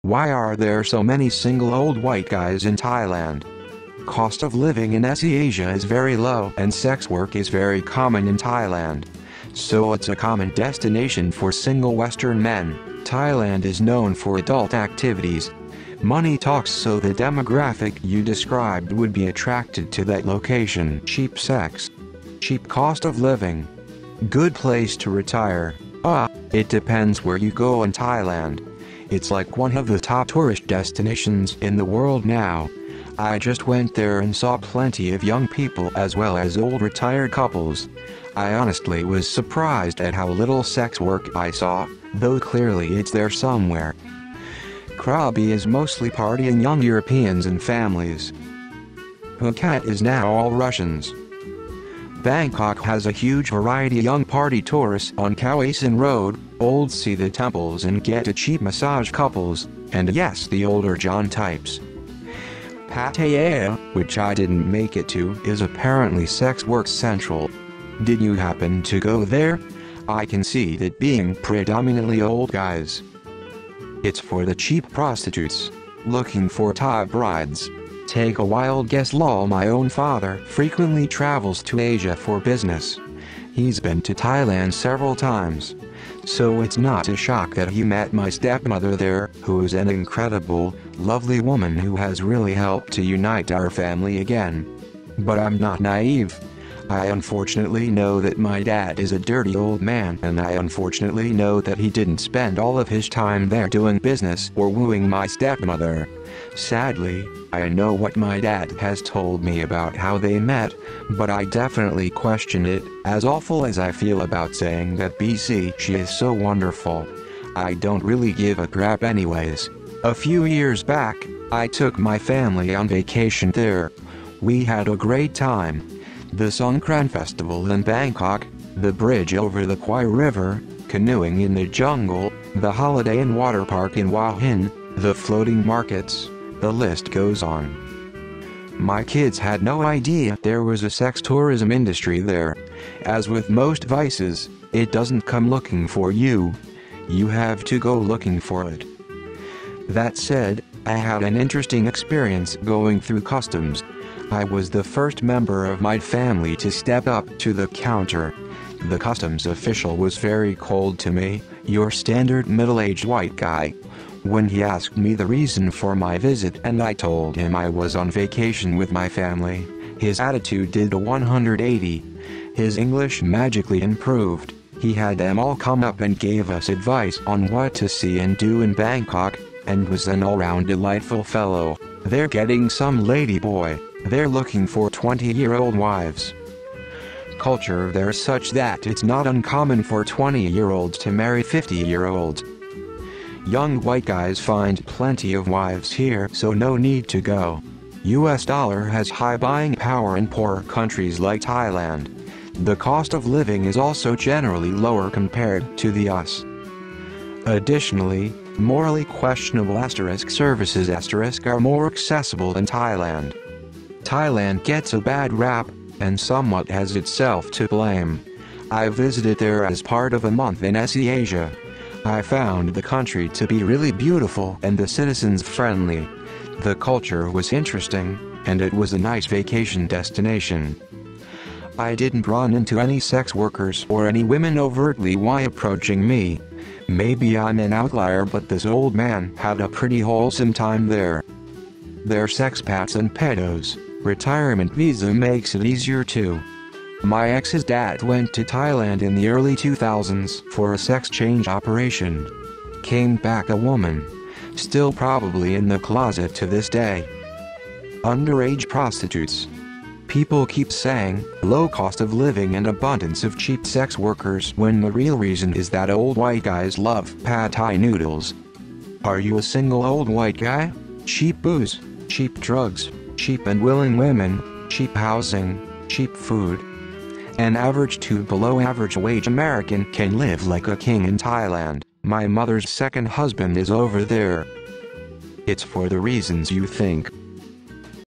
Why are there so many single old white guys in Thailand?
Why are there so many single old white guys in Thailand? the question you all have been asking for. I have been asked this question so many times that I decided to make a video about it. I hope you enjoy it and if you have any questions, please leave them in the comments below.